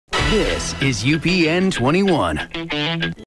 KTXA Station ID 2005
This is KTXA's station ID from 2005
KTXA is located in Fort Worth, TX